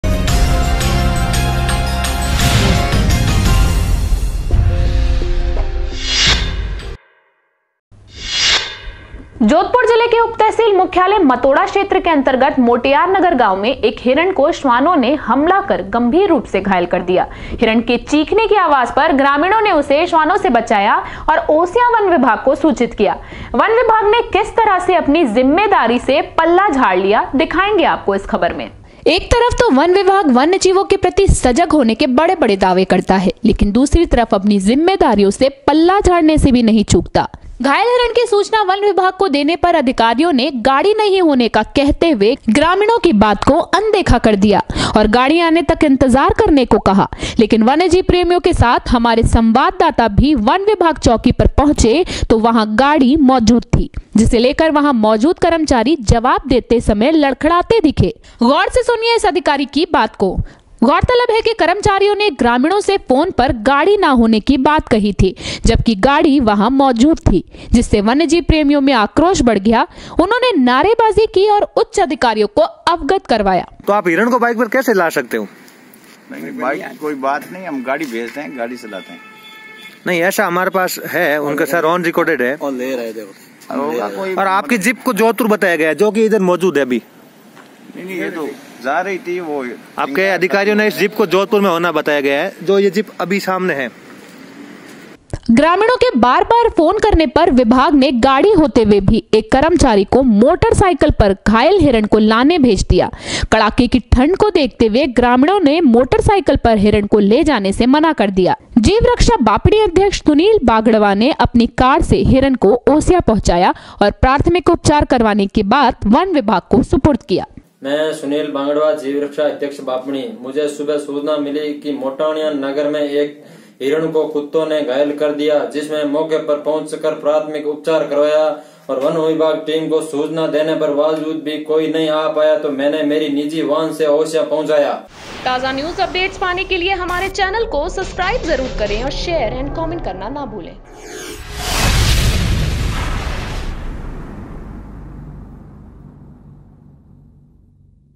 घायल हिरण को लेकर वन चोकी पहुंचे वन्यजीव प्रेमी, तो खुली वनविभाग की पोल देखिये एक शानदार खबर...
जोधपुर जिले के उपतहसिल मुख्यालय मतोडा क्षैत्र के अन्तर्गत मोटानियानगर गांव में एक हिरन को शवानों ने हमला कर गंभीर रूप से घायल कर दिया। हिरण के चिखने की आवाज पर ग्रामीणों ने उसे श्वानो से बचाया ओर ओसियां वन विभाग को सूचित किया। वन विभाग ने किस तरह से अपनी जिम्मेदारी से पल्ला झाङ लिया दिखाएंगे आपको इस खबर में.